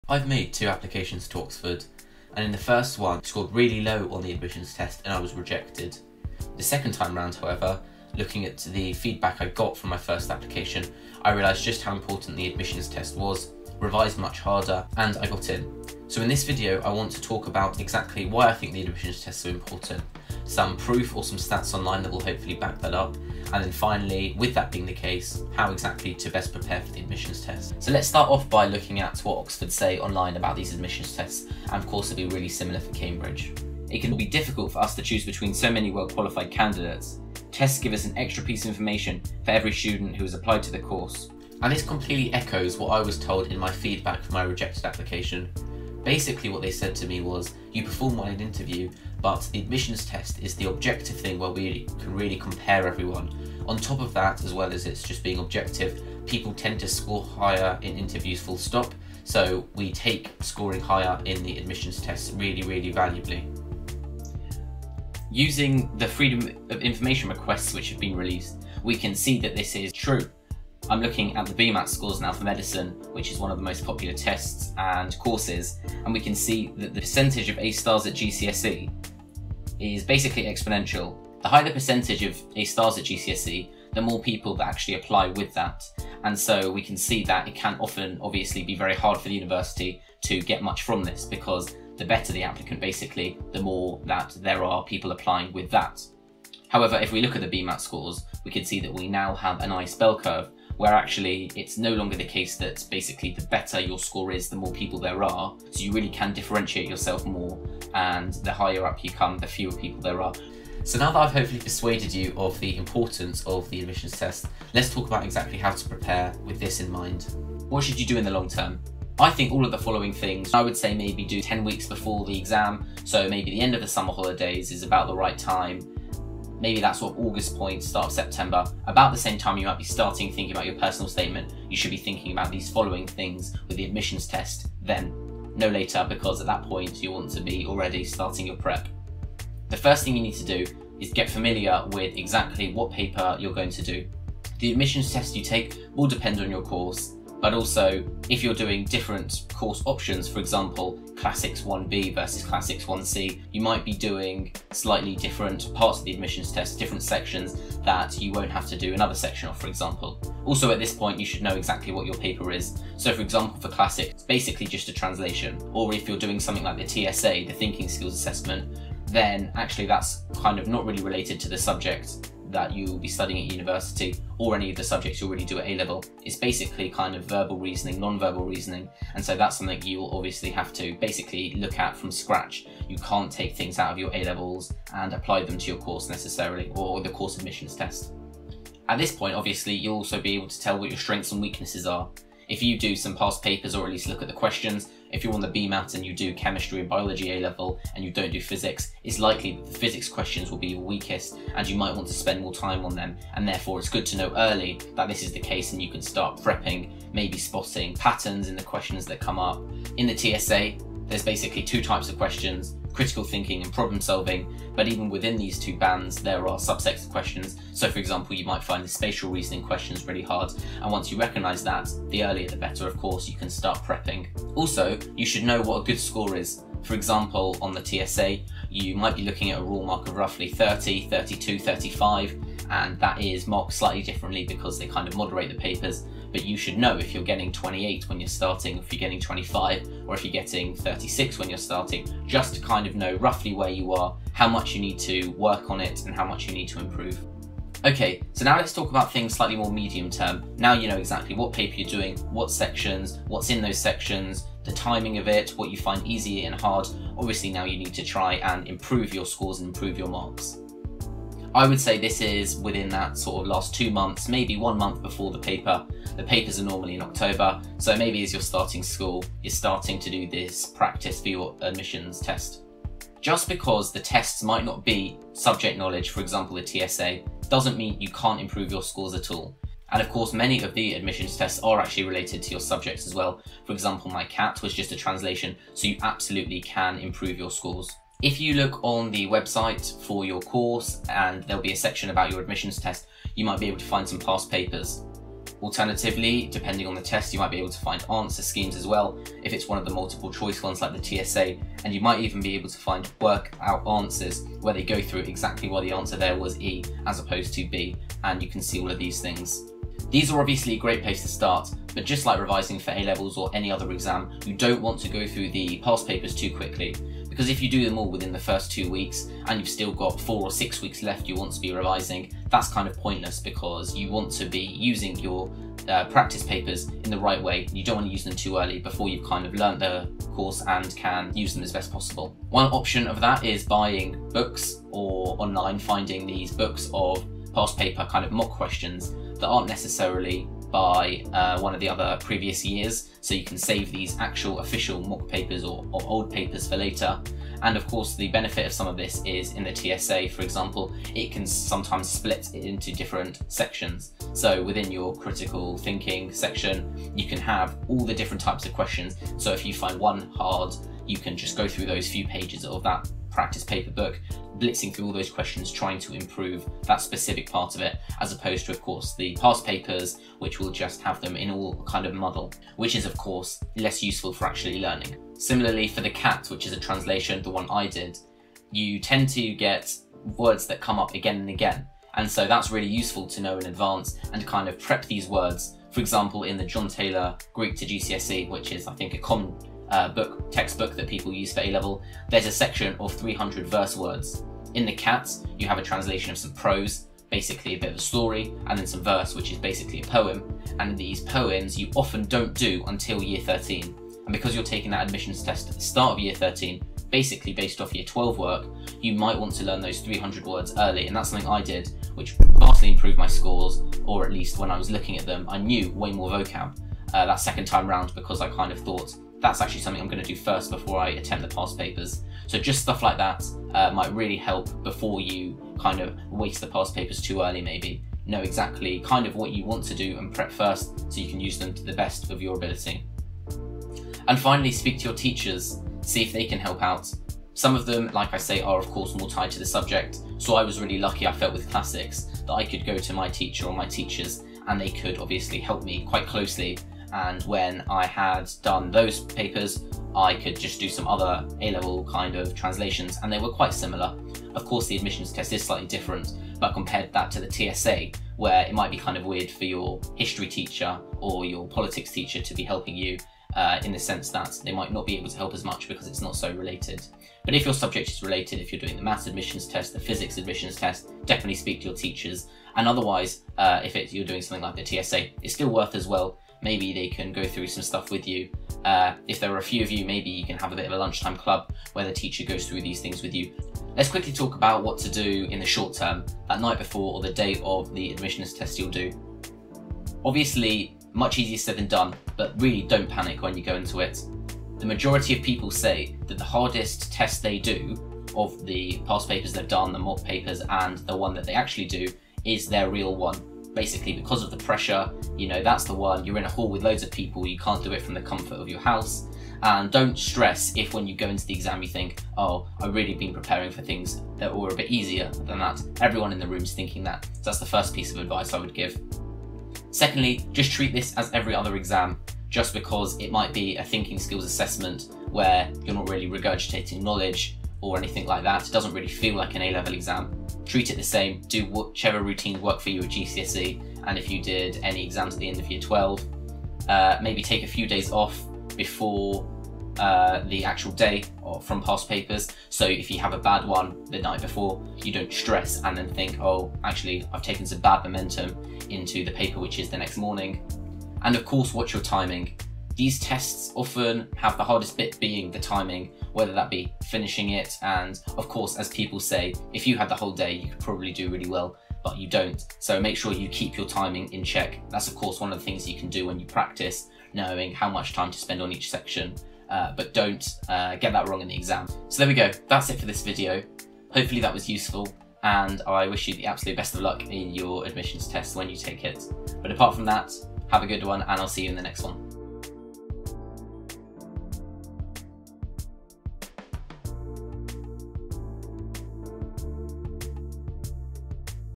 Oxford University Admission Tests: How to smash them
I have taken two admissions tests, one when I was rejected and one where I was accepted. Understanding how important admissions tests are to an application the second time round, I prepared much better and got a much better result which was, in my opinion, the single biggest reason as to why I got a place.

This video goes through why admissions tests are so important for Oxford applications (or Cambridge applications), some stats to strengthen prove this, and then how to best prepare.

Timestamps:
0:00 - Intro
0:27 - Proof that tests are crucial
1:58 - Long Term
4:11 - Medium Term
8:03 - Short Term